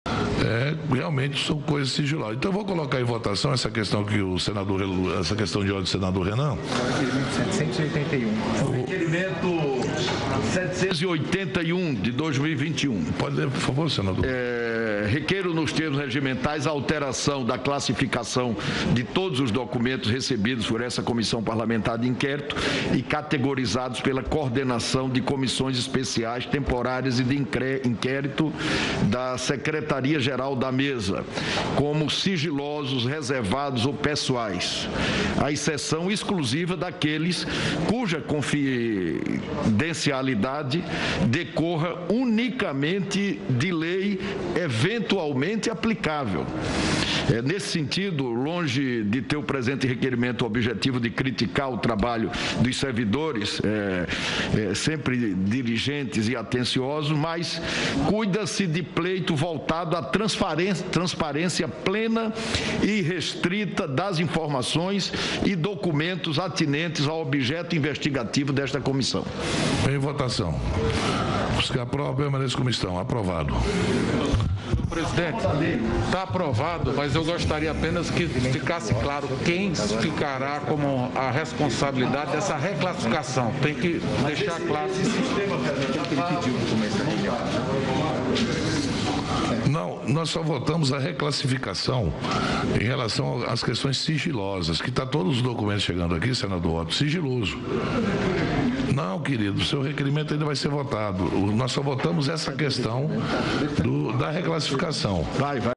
CPI aprova reclassificação de documentos sigilosos
A CPI da Covid aprovou nesta quarta-feira (9) a reclassificação de documentos sigilosos afim de trazer transparência aos dados que foram recebidos pela comissão. Segundo Renan Calheiros, relator da CPI, a ideia é deixar como sigilosos, apenas dados como informações bancárias, fiscais e assuntos de segurança nacional e de interesse de Estado.